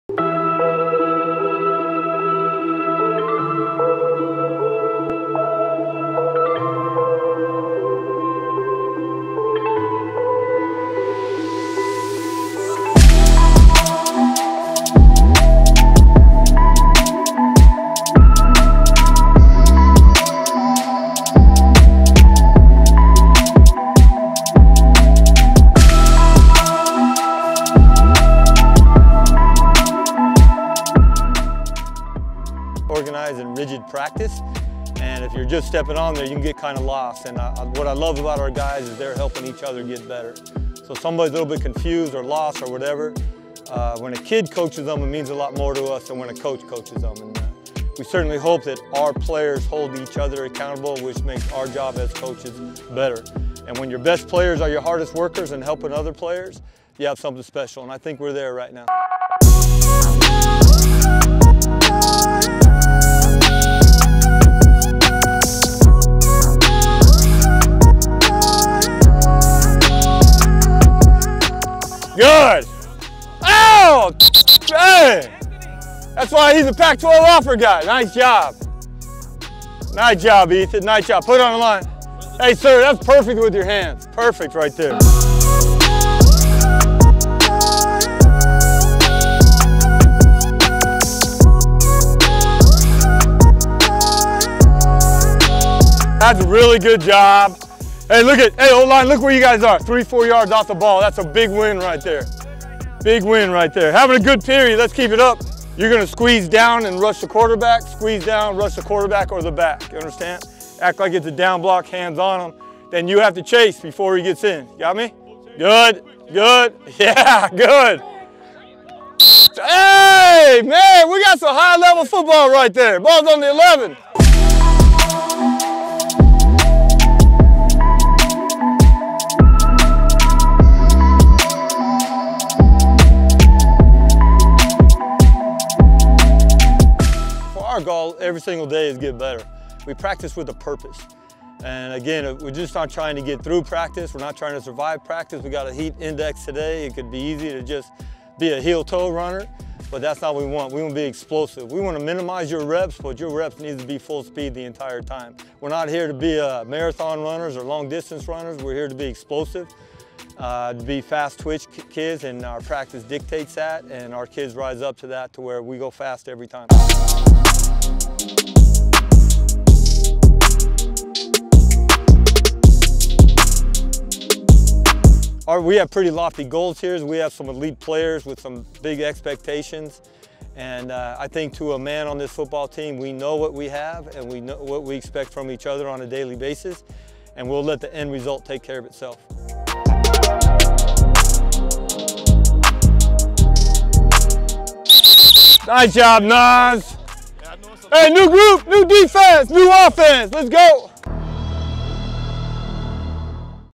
Los Alamitos High School Football 🔥 EXCLUSIVE Look At ELITE Orange County Program ⭐ D1 Prospects
Stars are out in full force during Los Alamitos High School Football summer training camp. With several Division-1 prospects and Power 5 college commits, the Griffins are poised to be among the most dominant programs in a loaded Orange County high school football landscape. Head Coach Ray Fenton previews the upcoming season and praises his team's ability to hold each other accountable and improve every day.